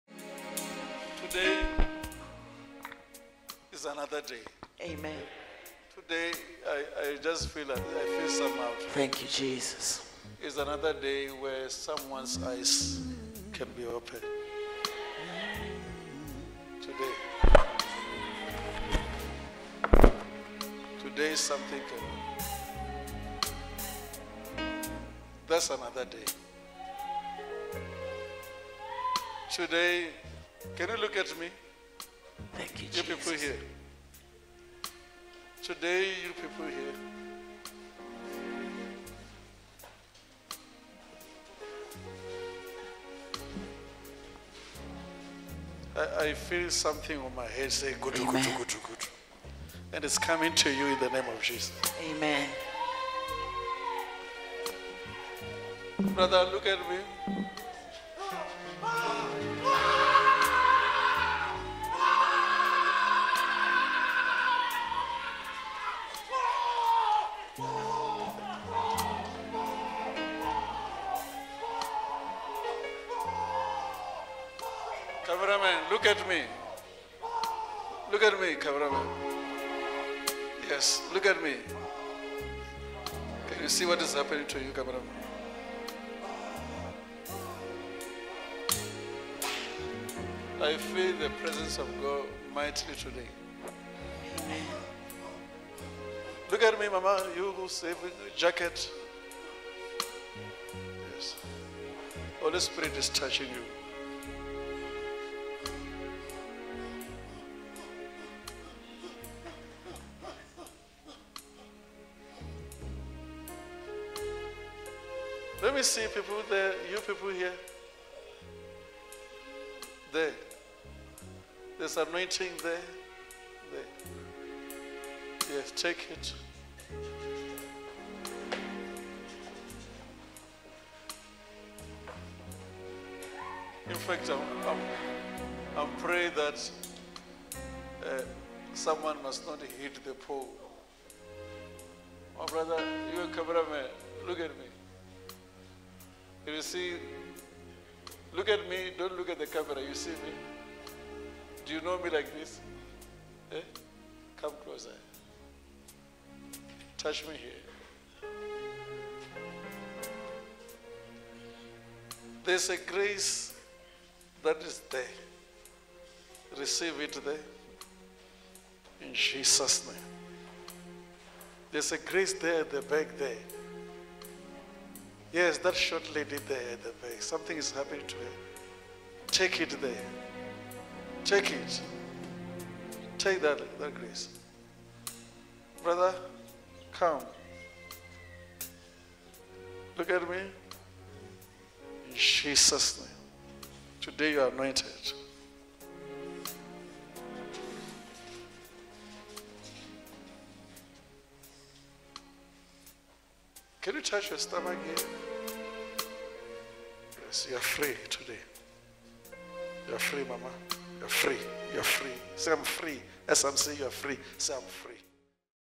ANOINTING TO OPEN YOUR EYES 👀👁 l Apostle BJ Makananisa
“Holy Spirit is touching you - I just feel somehow, where someone’s eyes can be opened.” – Apostle BJ Makananisa